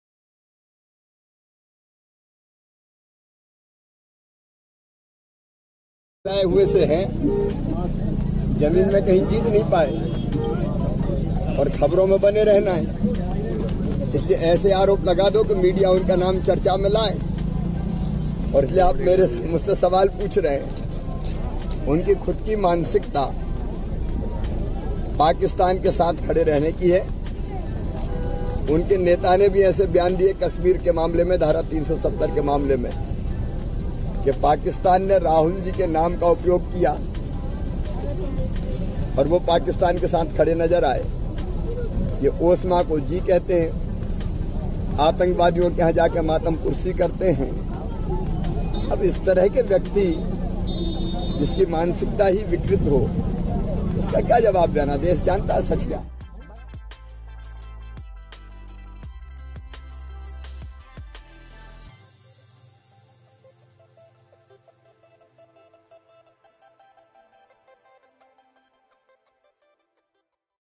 Digvijay Singh की मानसिकता Pakistan के साथ खड़े रहने की हैः Shivraj Singh
दिग्विजय सिंह की खुद की मानसिकता पाकिस्तान के साथ खड़े रहने की है। उनके नेता राहुल जी ने भी धारा 370 के मामले में ऐसा बयान दिया कि उसका उपयोग पाकिस्तान ने किया। ऐसे नेता की बात का क्या जवाब देना, देश सच जानता है